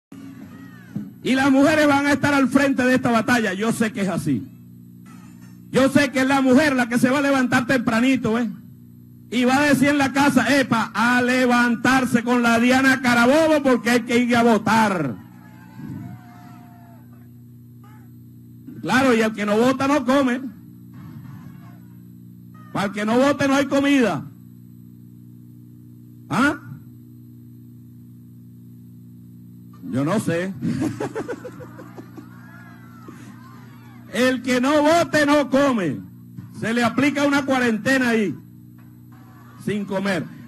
Cabello: El que no vote, no come, se le aplica una cuarentena ahí sin comer
ND / 30 nov 2020.- El primer vicepresidente del Psuv, Diosdado Cabello, lanzó este lunes una amenaza a quienes decidan no ir a votar en las elecciones parlamentarias del próximo 6 de diciembre, al señalar que quien no vote “no come”.

“A levantarse con la diana de Carabobo porque hay que ir a votar. Claro y el que no vota, no come, para el que no vote no hay comida, yo no sé… El que no vote no come, se le aplica una cuarentena ahí sin comer”, expresó Cabello en un video difundido en Twitter por el embajador/AN en EEUU, Carlos Vecchio.

Ante esto, Vecchio respondió que a la “dictadura de Maduro solo le queda la amenaza como "movilizador" del fraude 6D. Cabello afirma que "no hay comida para quien no vote". Pero Diosdado, si no hay comida desde hace tiempo. Más de 9 millones venezolanos están en inseguridad alimentaria por crisis que ustedes crearon”.

“Más de 5 millones de venezolanos emigraron buscando alimentos y recursos fuera de Venezuela porque ustedes arrasaron con todo. Ni hablemos de la desnutrición infantil y la dolarización de facto de la economía que agrava la situación. ¿Con qué amenazas entonces?”, concluyó Vecchio.